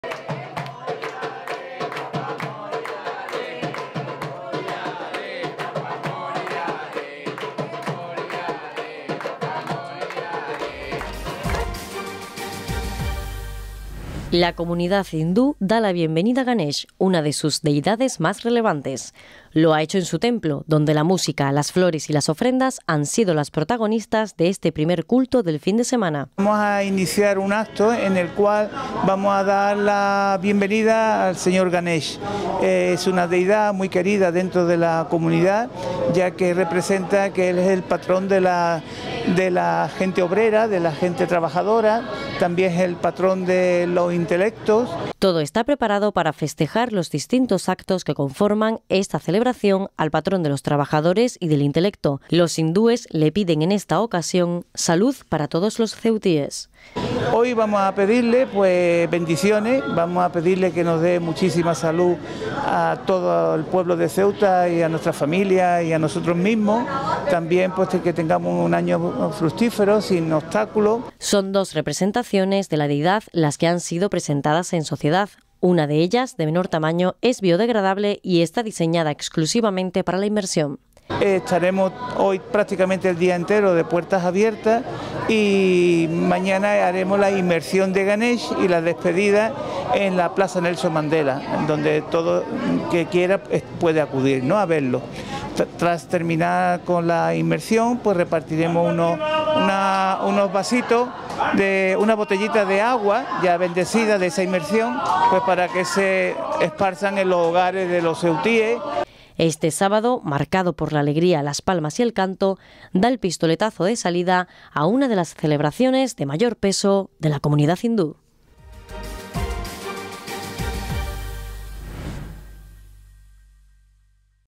La comunidad hindú ve en primicia a Ganesh
Después de un mes de espera la comunidad hindú de Ceuta al fin ve a Ganesh. Tras permanecer oculto bajo un manto, recibe a la deidad a ritmo de palmas y de un mantra. Una celebración significativa en la que se ensalza a esta divinidad.

Redacción y locución: María Valverde
Imagen y edición: Imad Lagmich